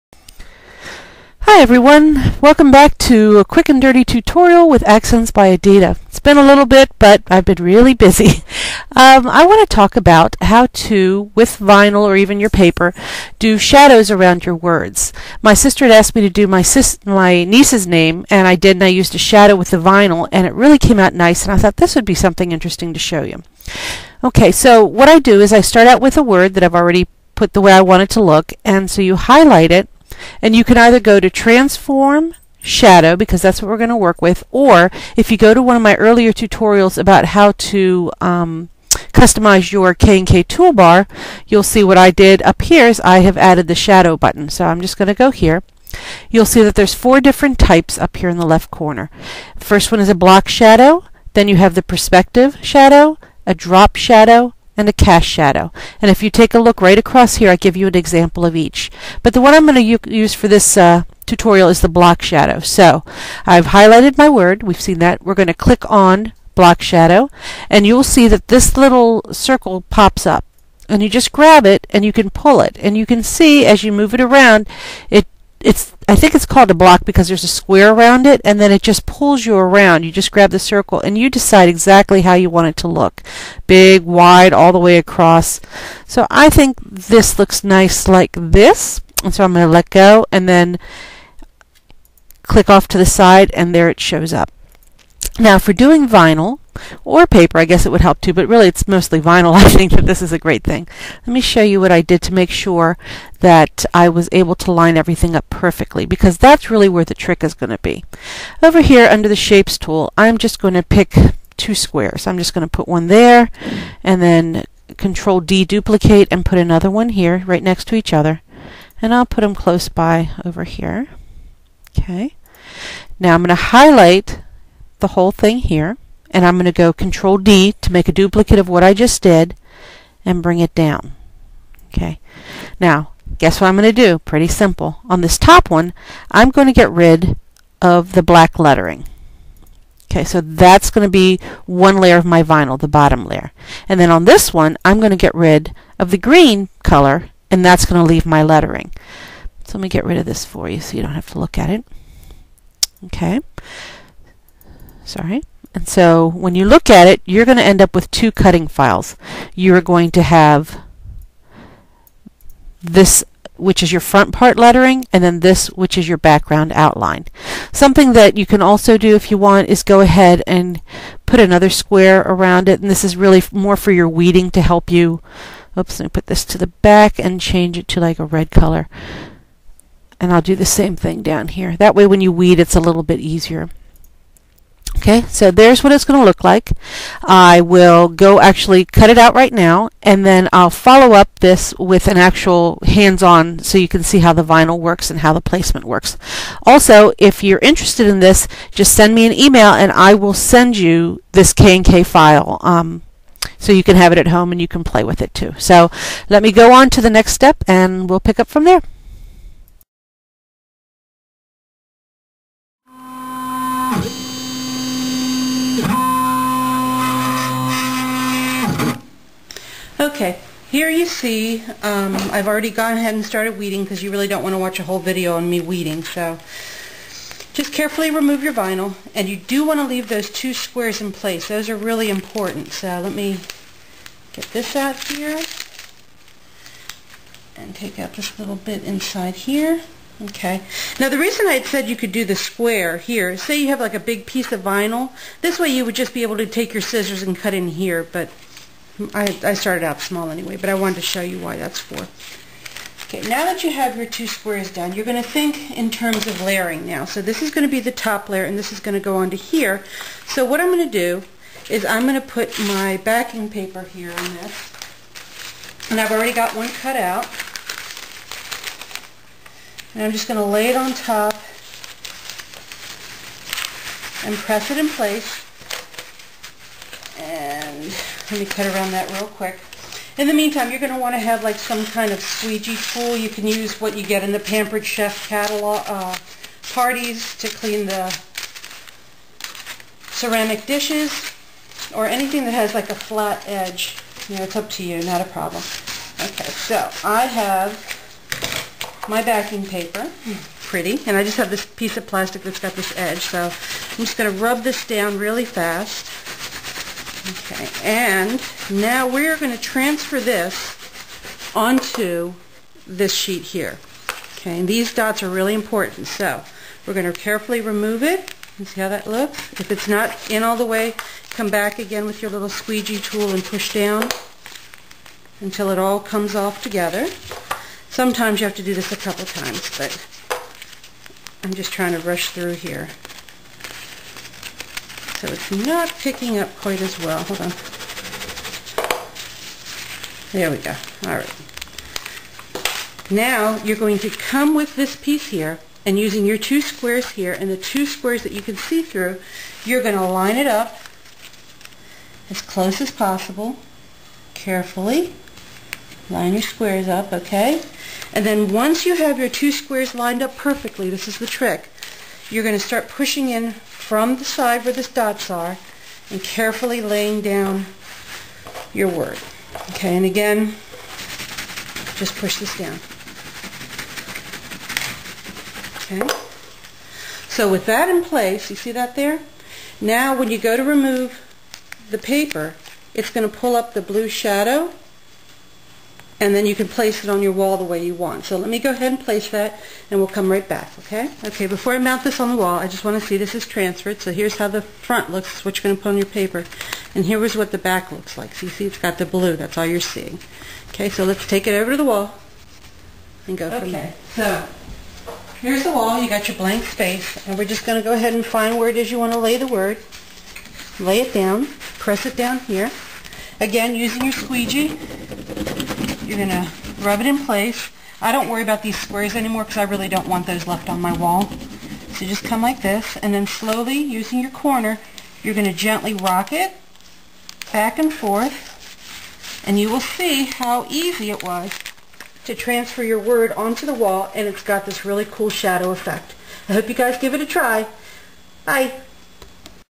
Klic-N-Kut KNK Adding Shadows to your Wall Vinyl Lettering
Welcome to another Accents By Eddita Quick and Dirty tutorials.  Please check out my blog for more videos and to catch up on my current news:
We all can cut out wonderful words or phrases for our walls etc using vinyl, but what if you want to add a shadow.  While this focuses on how to create it in your KNK software, the technique (double squares) would work just as well in whatever program you are using to create lettering with other machines.
For a free copy of the file I used in the demonstration, just come to my blog.  There is a link for you to download the extension you need (.KNK, .AI, .GSD, .SVG & .WPC)